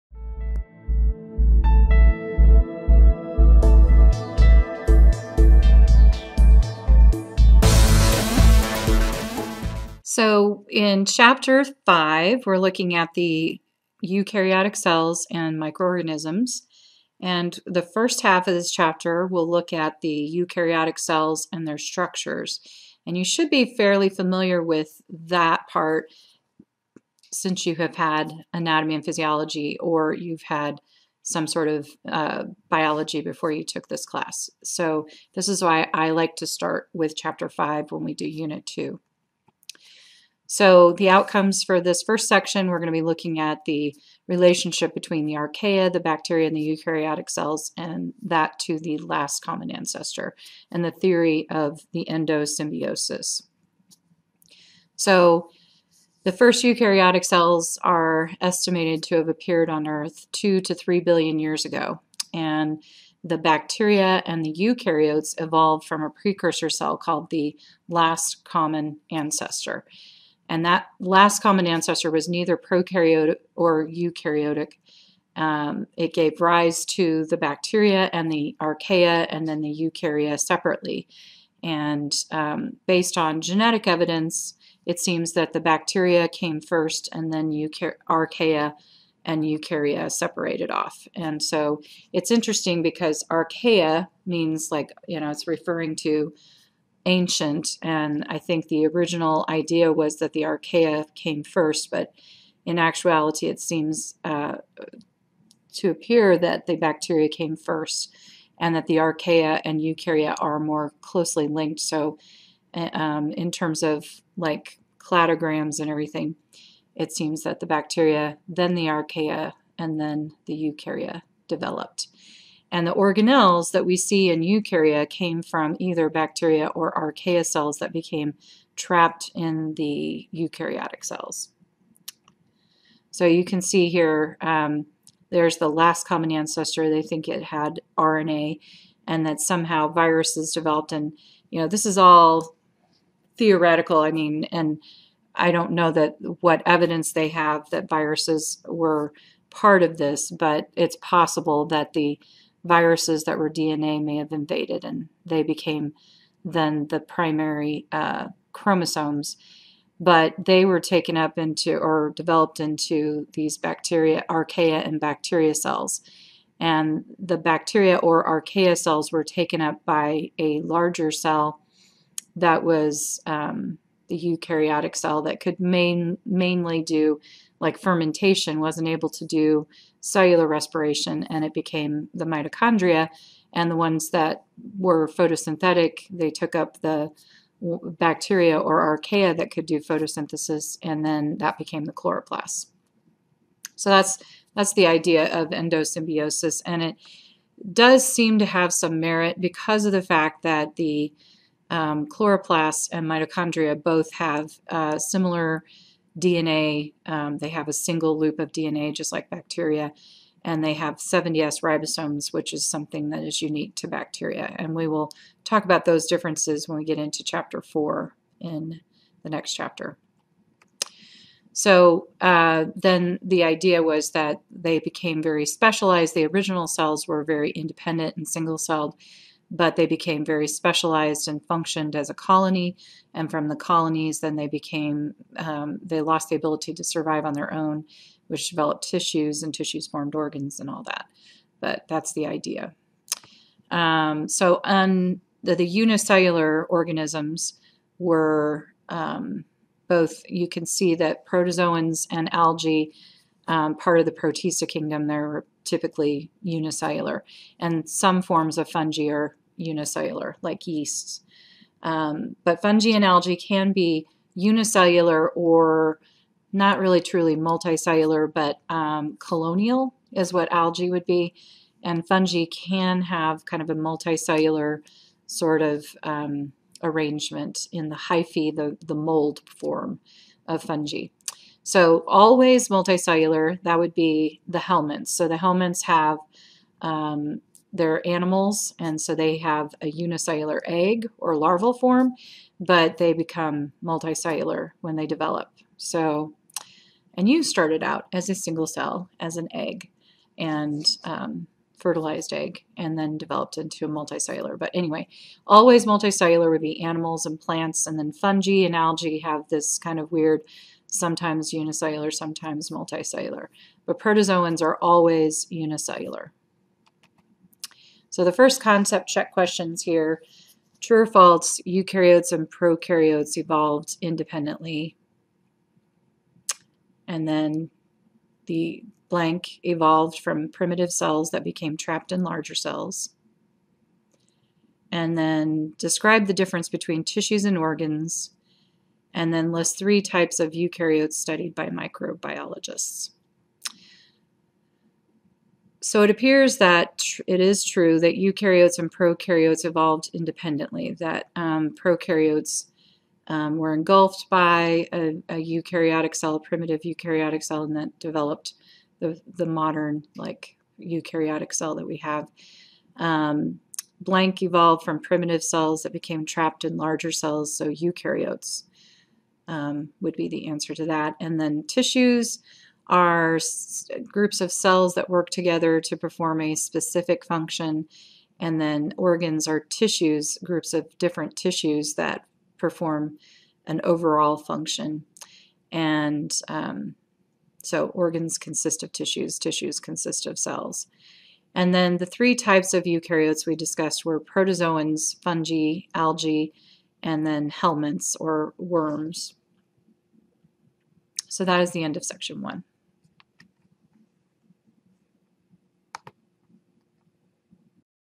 Unit 2 - Chapter 5-1 sp22
Section 5-1 from Cowan's 6th edition of Microbiology: Asystems Approach. This discusses the origins of Eukaryotic cells and the endosymbiosis theory.